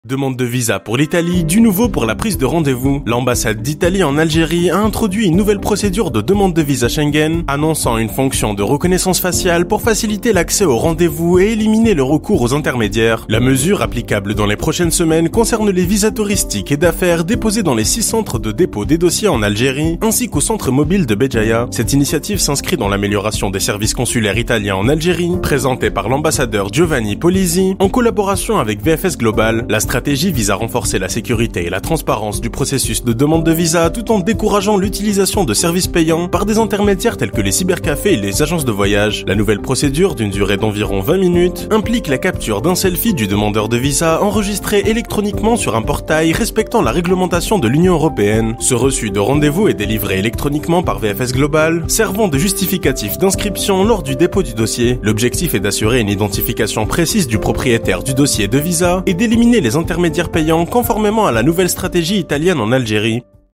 Demande de visas pour l'Italie : du nouveau pour la prise de rendez-vous
Du nouveau pour les demandeurs algériens de visas Schengen pour l'Italie. Dans un communiqué publié, ce jeudi 7 décembre, l'ambassade d'Italie en Algérie a annoncé une nouvelle procédure de « reconnaissance faciale » pour « faciliter l'accès aux rendez-vous de demande de visa » et à « éliminer le recours aux intermédiaires » par les demandeurs de visa.